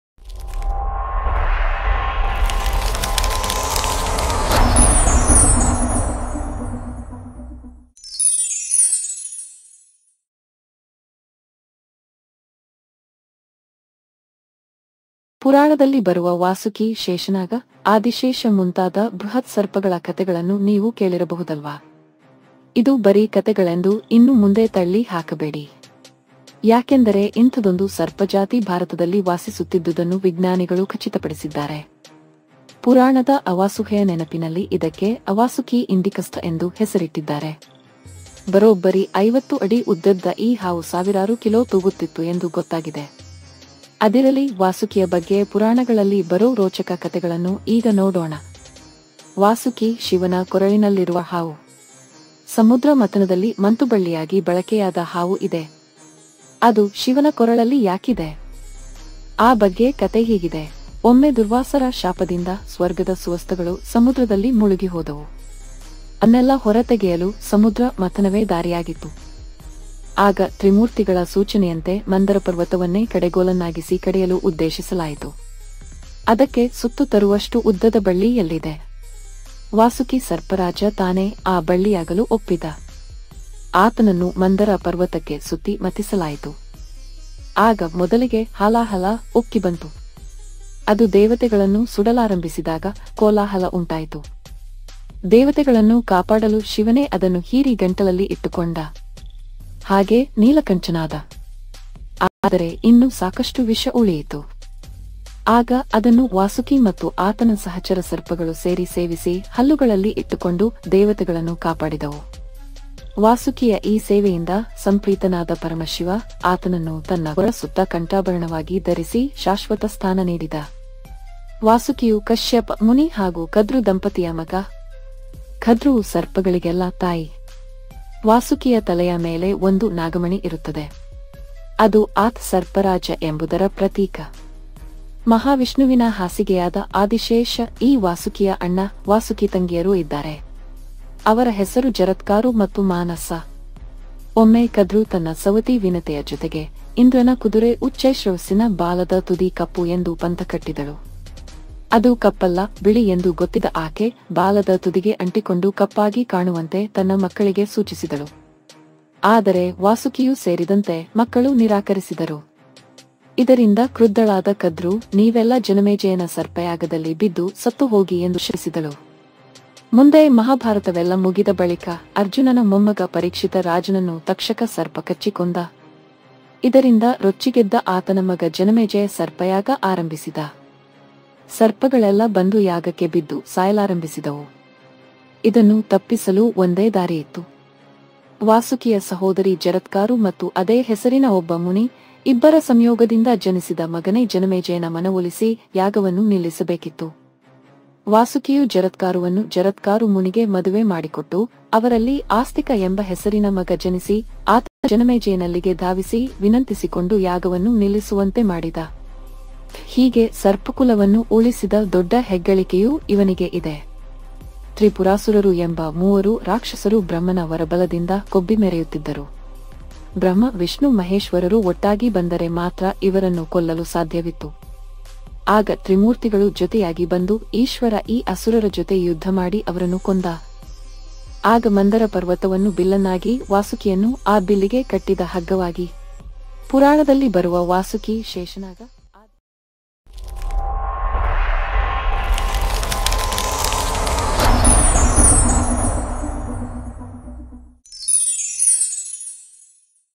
ಜಗತ್ತಿಗೆ ಇರುವುದೊಂದೆ ವಾಸುಕಿ ಸರ್ಪ. ಇದು ದೇವರ ಹಾವು. Target Kannada News
There is only one snake in the world. It is the snake of God.